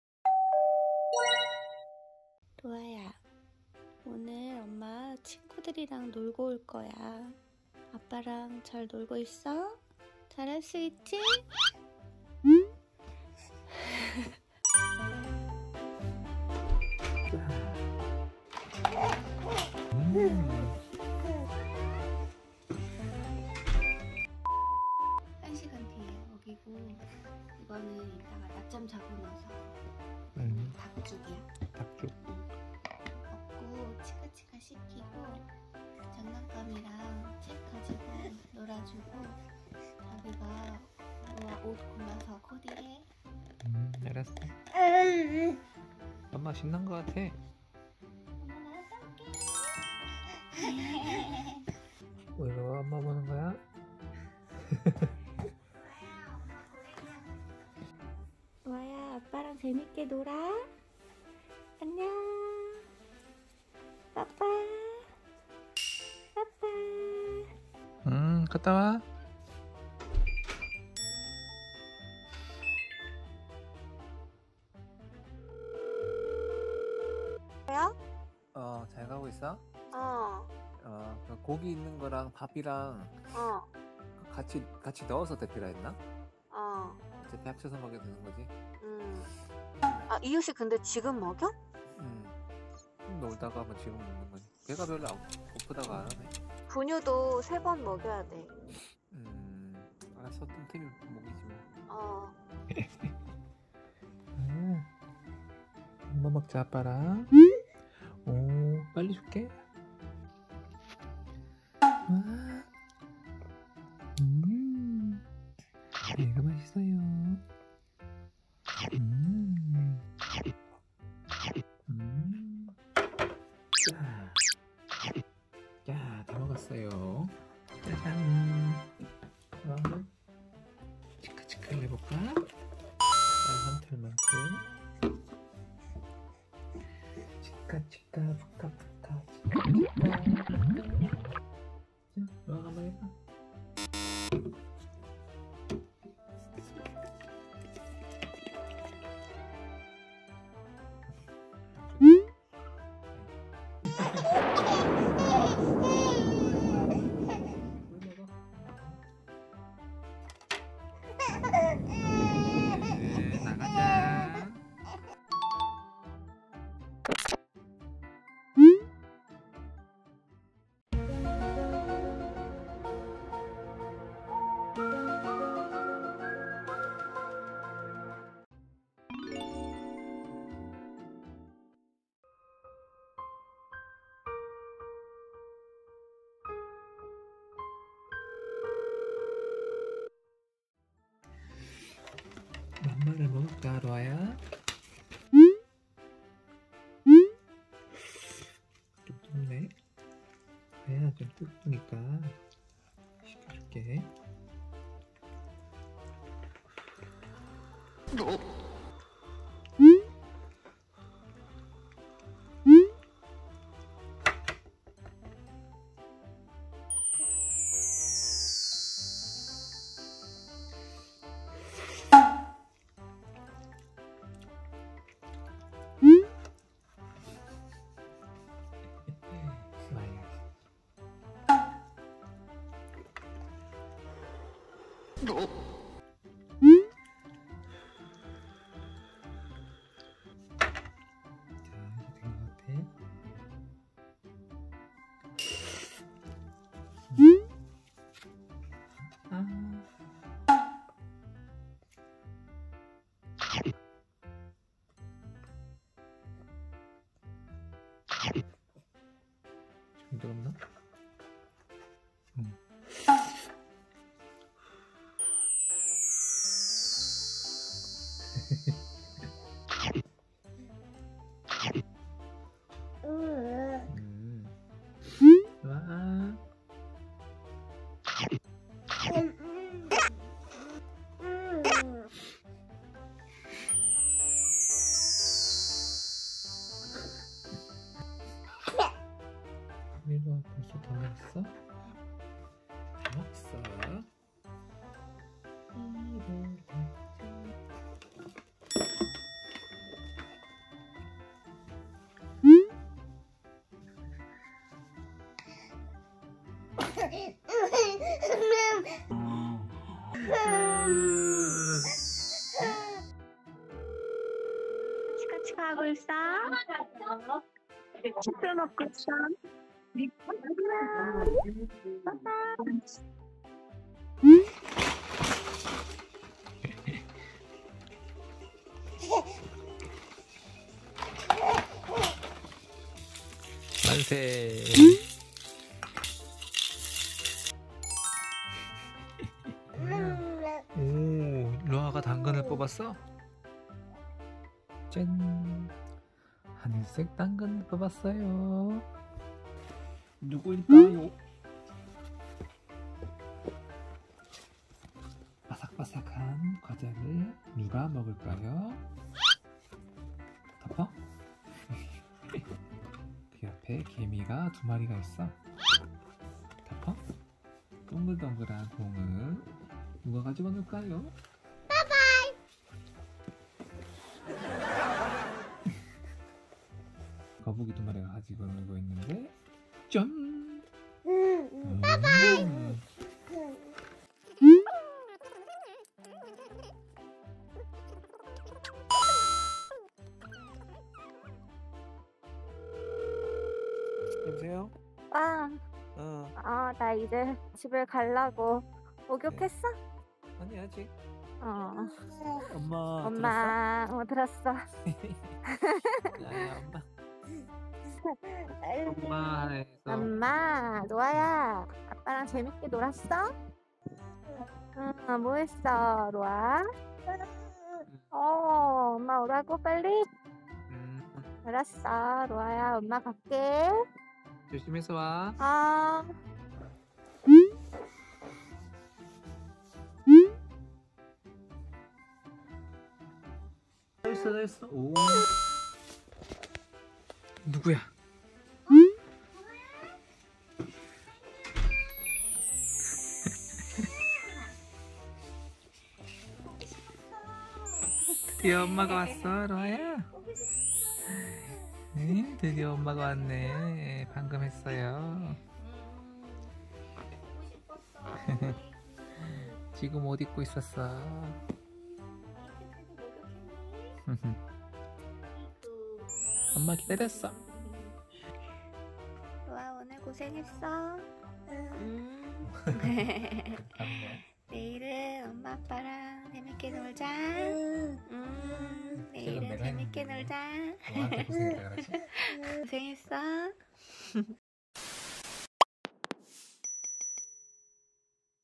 (SUB) 아빠의 독박육아 - 자유부인 끝나고 집에 온 엄마가 경악한 사연
2022년 4월 7일생 정로아(태명: 달링)의 221217 성장기록입니다.
Baby RoA's daily life
Born on April 7th, 2022 / name: RoA Jeong / fetus nickname: Darling

✅ 이 영상은 뮤팟이 제작한 배경음악을 사용했습니다🐱